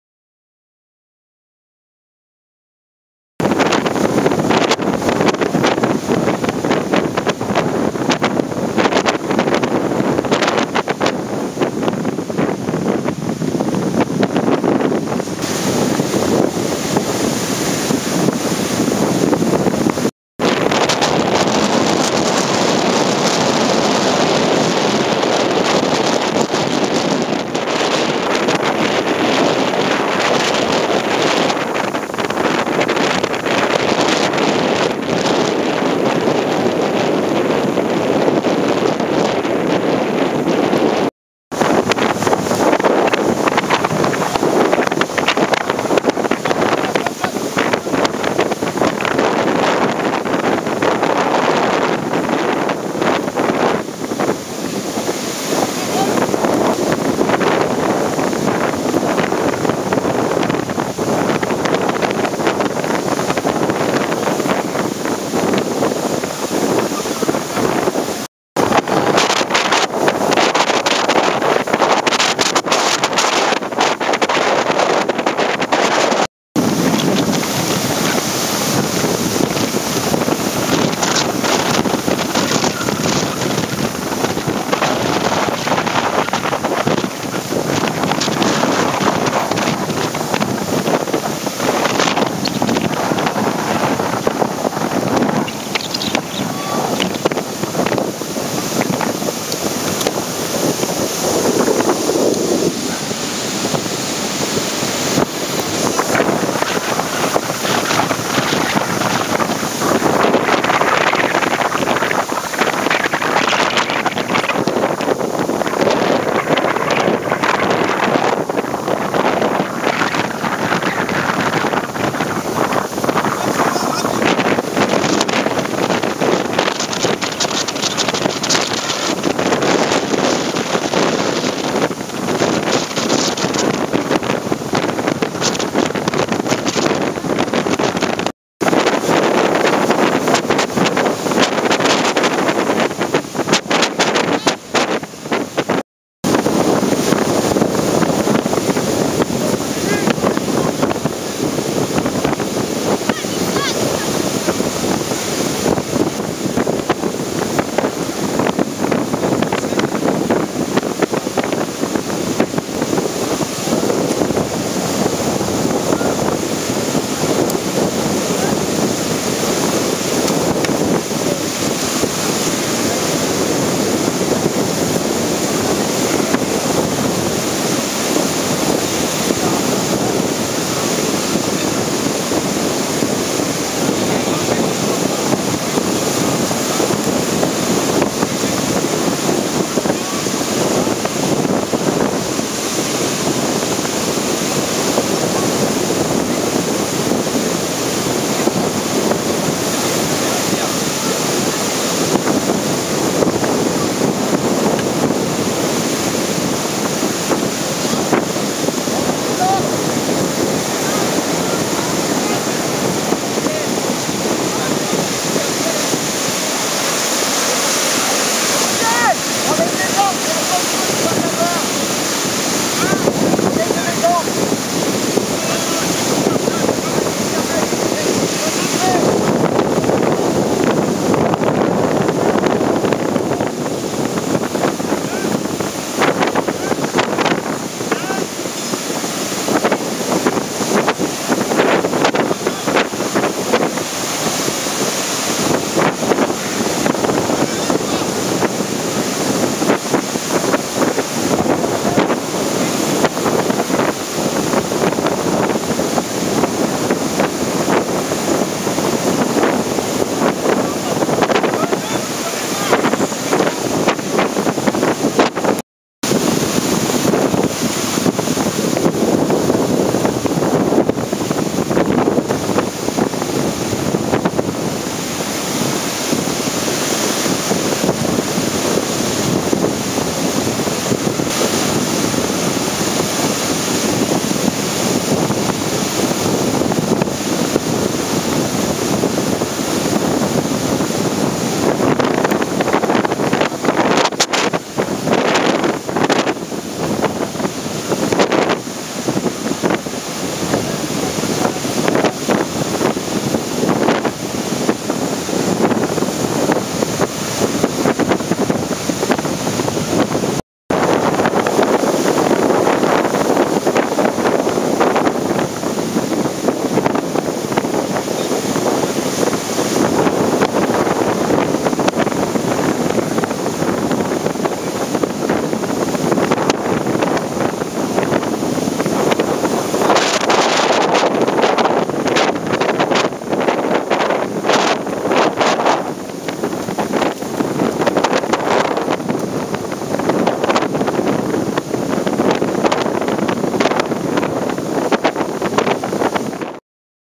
2012 04 30 EUROCAT Vedette echouee plage extraction logo FFSS HD 720
Vedette de l'organisation EUROCAT échouée sur une plage de CARNAC dans la nuit du 29 au 30 avril 2012. Extraction par les sauveteurs de la FFSS (Fédération Française de Sauvetage Secourisme) et le club de voile de Carnac. Zodiac avec un moteur de 20 Chevaux